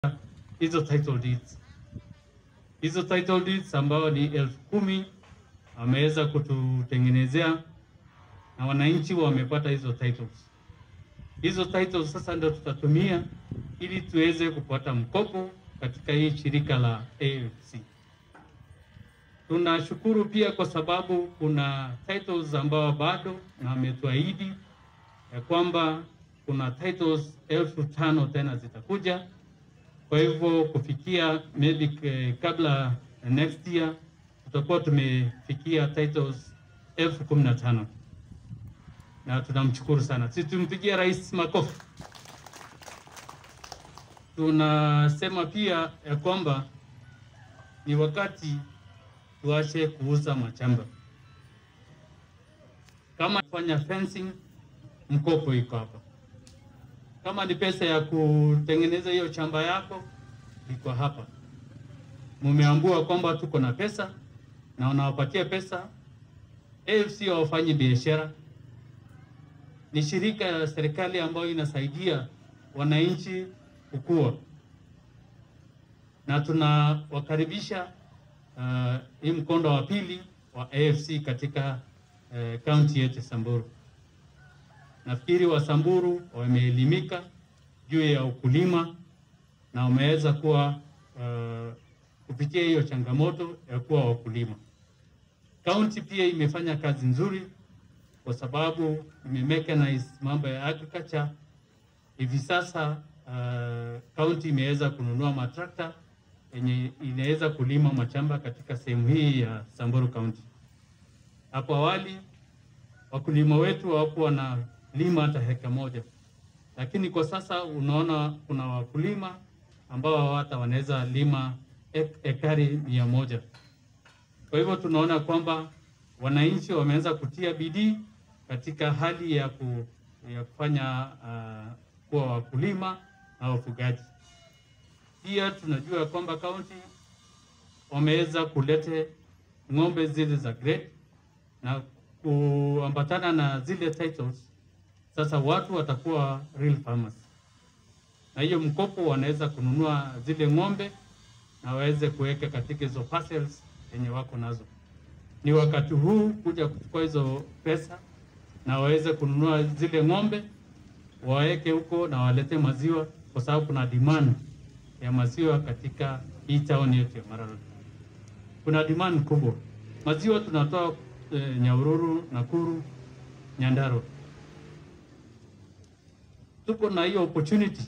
Samburu Deputy Governor Julius Leseeto urges locals to use title deeds to acquire advance loans
Samburu Deputy Governor Julius Leseeto urges locals to use title deeds to acquire advance loans from the Agricultural Finance Corporation (AFC) to realize high production of projects.